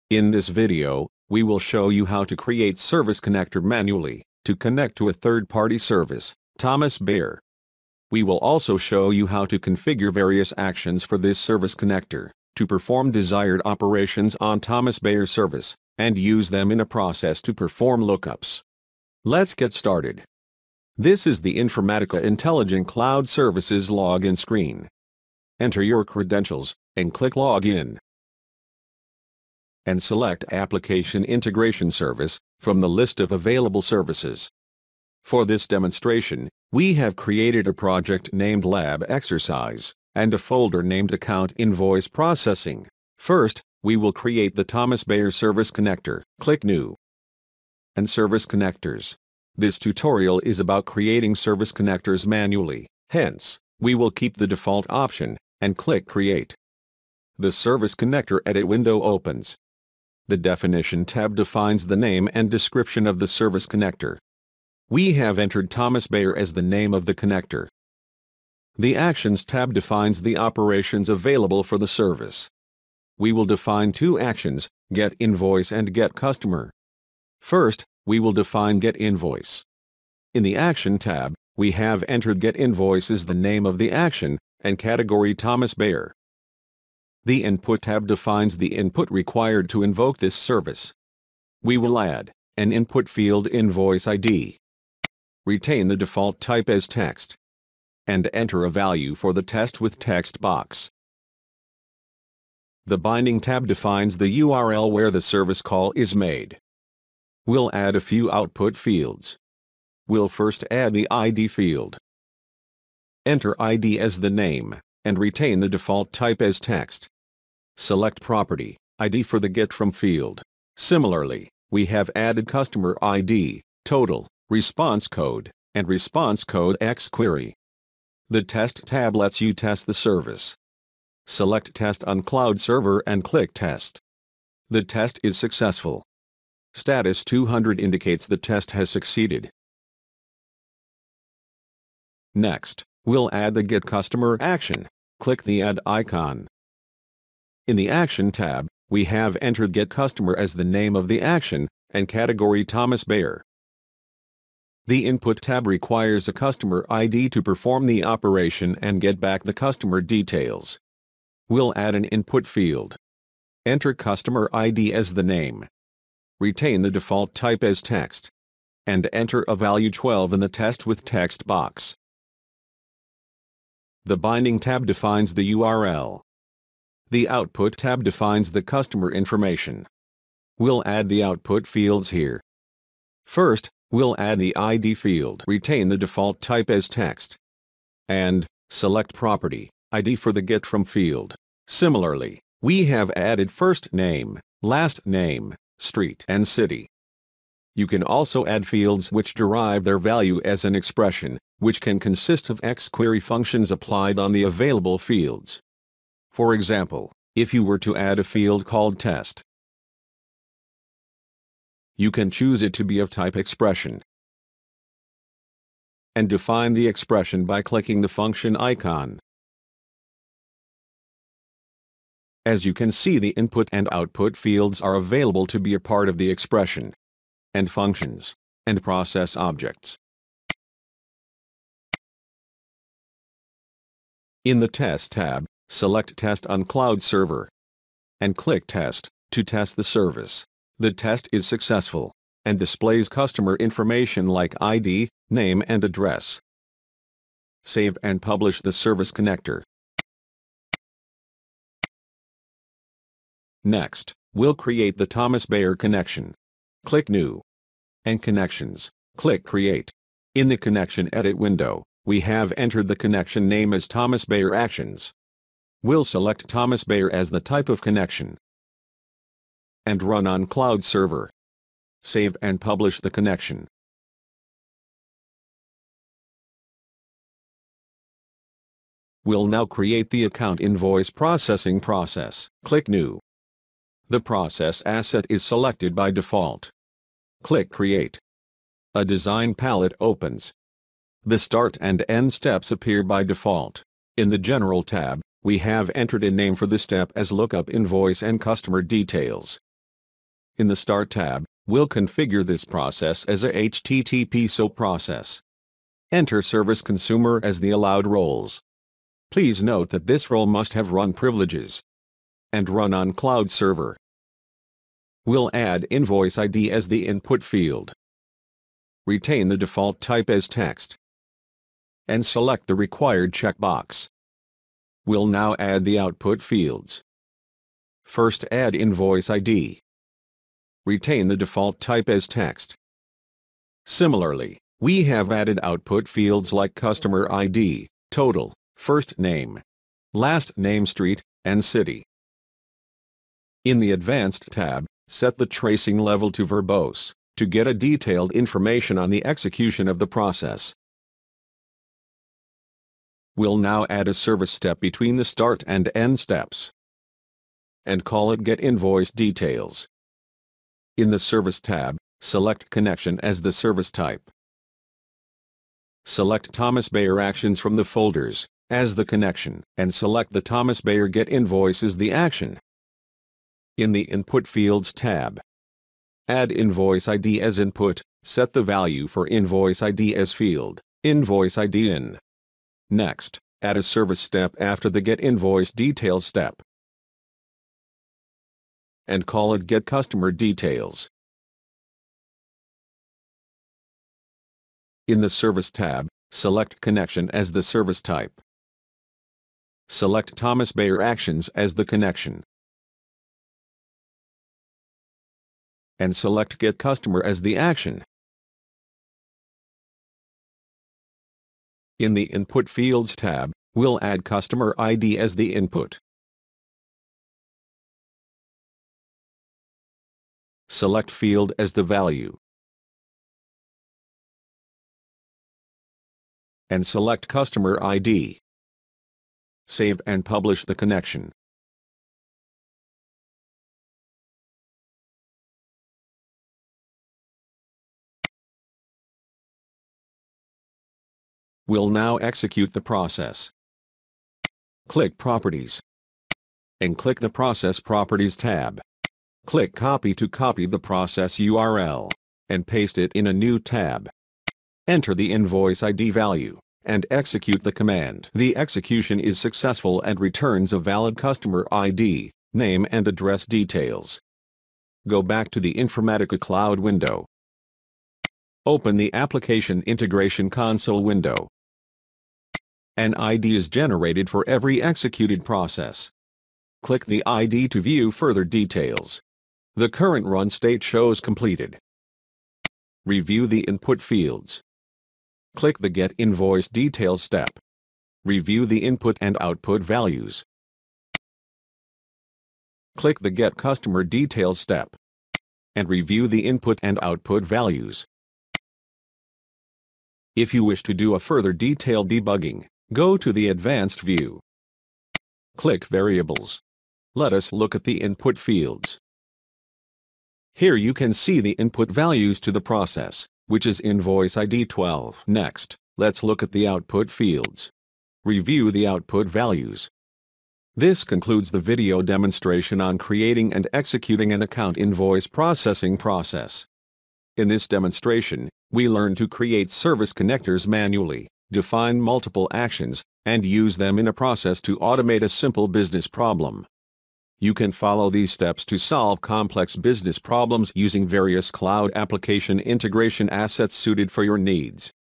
How to design Cloud Application Integration Process using Third-Party WebServices - Part 2
Welcome to Cloud Application Integration Enablement Videos.

This video demonstrates the following:

1. Creating Service connector manually using API Documentation
2. Connecting to a third-party service using REST
3. Lookup between two services